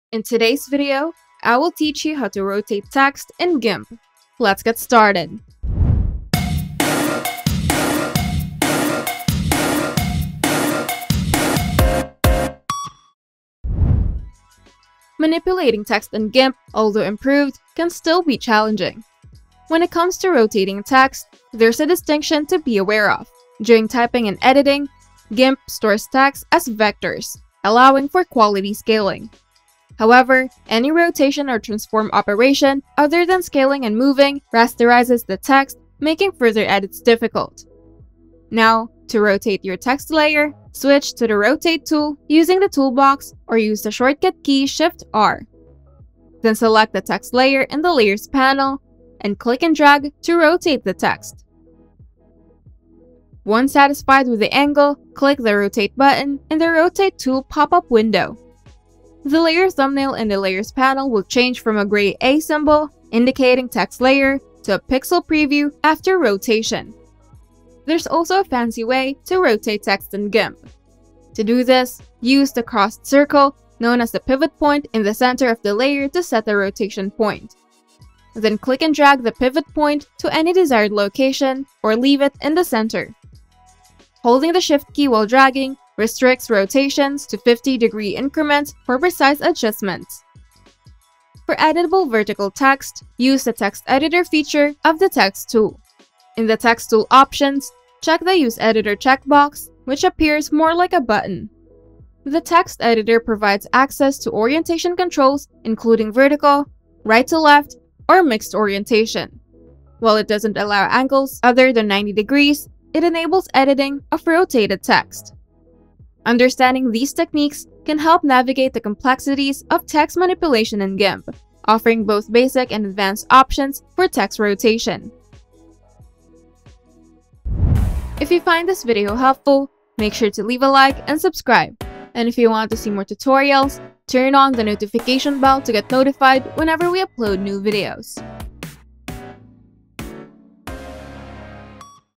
How to ROTATE TEXT in GIMP (Rotate, Change Orientation, Flip, etc.)
Discover how to rotate text horizontally, vertically, or diagonally; change the orientation, flip, and other ways to manipulate text in Gimp!